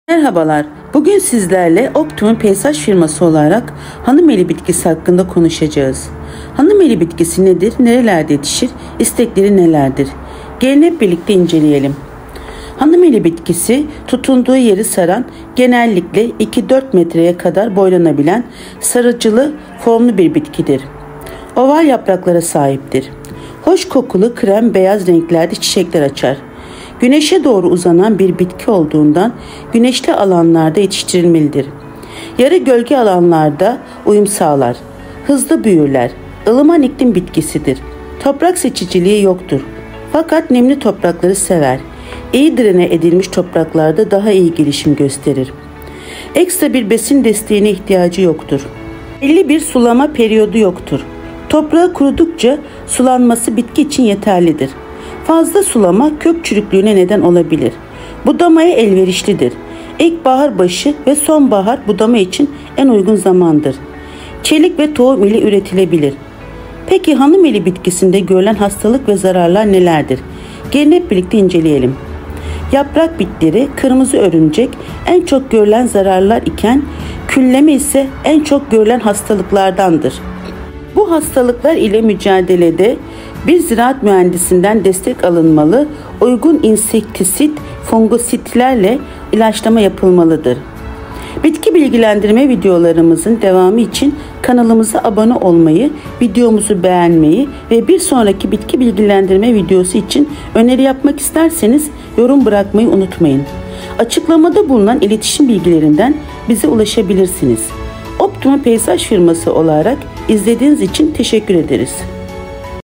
BİTKİ ANSİKLOPEDİSİ | HANIMELİ - LONİCERA NEDİR? İSTEKLERİ NELERDİR? HANIMELİ BAKIMI NASIL YAPILIR?
Optimum Peyzaj firması olarak Bitki Ansiklopedisi serimizin otuzuncu videosunda Hanımeli bitkisi nedir,nerelerde yetişir, istekleri nelerdir bu konulardan bahsettik.

Hanımeli - Lonicera
Merhabalar, bugün sizlerle Optimum Peyzaj firması olarak hanımeli bitkisi hakkında konuşacağız. Hanımeli bitkisi nedir, nerelerde yetişir, istekleri nelerdir gelin hep birlikte inceleyelim.
Hanımeli bitkisi tutunduğu yeri saran, genellikle 2-4 metreye kadar boylanabilen sarılıcı formlu  bir bitkidir.
Oval yapraklara sahiptir.
Hoş kokulu krem, beyaz renklerde çiçekler açar.
Güneşe doğru uzanan bir bitki olduğundan güneşli alanlarda yetiştirilmelidir. Yarı gölge alanlara da uyum sağlar. Hızlı büyürler.
Ilıman iklim bikisidir. Toprak seçiciliği yoktur fakat nemli toprakları sever. İyi drene edilmiş topraklarda iyi gelişim gösterir. Ekstra bir besin desteğine ihtiyaç duymaz.
Belli bir sulama periyodu yoktur. Toprağı kurudukça sulanması bitki için yeterlidir. Fazla sulama kök çürüklüğüne neden olabilir.
Budamaya elverişlidir. İlkbahar başı ve sonbahar budama için en uygun zamandır.
Çelik ve tohum ile üretilebilir.
Peki hanımeli bitkisinde görülen hastalık ve zararlılar nelerdir gelin hep birlikte inceleyelim.
Yaprak bitleri, kırmızı örümcek en çok görülen zararlılar iken, külleme ise en çok görülen hastalıklardır.
Bu hastalıklar ile mücadelede bir ziraat mühendisinden destek alınmalı, uygun insektisit-fungusitlerle ilaçlama yapılmalıdır.

BİZE ULAŞIN:
•ADRES: Çamlıçay Mah. Mareşal Fevzi Çakmak Bulvarı No:7 Urla/İZMİR